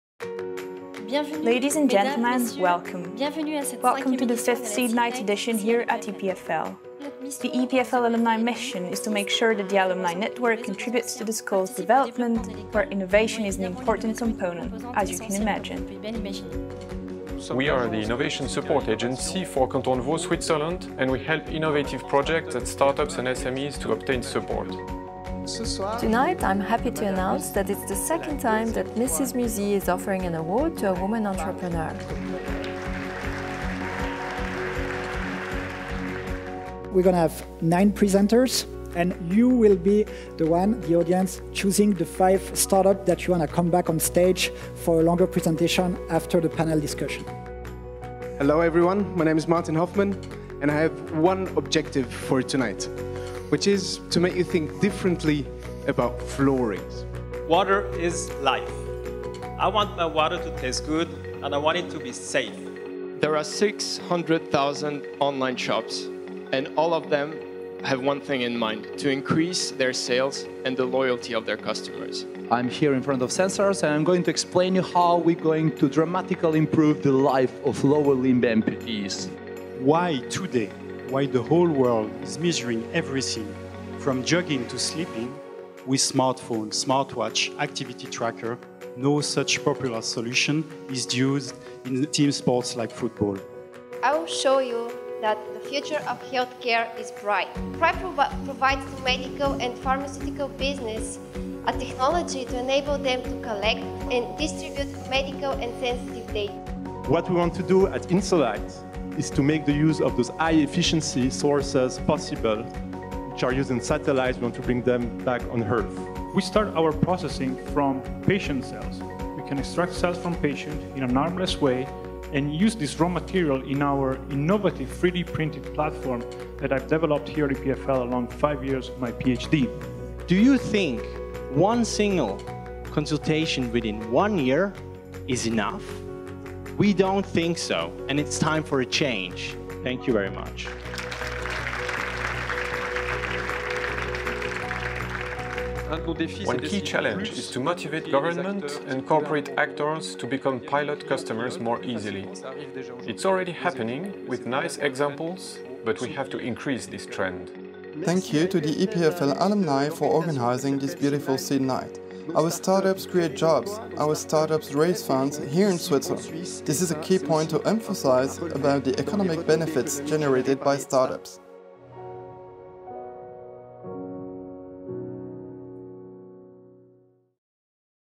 EPFL Alumni Seed Night 2016, ENGL.
The EPFL Alumni Seed Night, which took place on the 21st of April 2016, was presented by Mrs. Annelies Garcia, Head of the EPFL Alumni and Pierre Dorsaz, in charge of External Relations & Innovation for the EPFL Alumni. The Seed Night is an annual start-up presentation bringing together the most promising entrepreneurs from EPFL and beyond, angel investors, mentors, industry leaders and scientists.
9 startups - bNovate, Insolight, Technis, Qloudlab, Morphodyne, FieldWiz, Sensars, PrYv, TasteHit - pitched in public at the Rolex Learning Center. The Isabelle Musy Prize  was awarded to Sandra Sulser for her projetct Biome.
Mr. Patrick Barbey, Innovaud's Director and Mr. Raphaël Conz, Economic promotion manager at the Office for Economic Affairs (SPECo) explain the startup environment in the canton of Vaud and in Switzerland.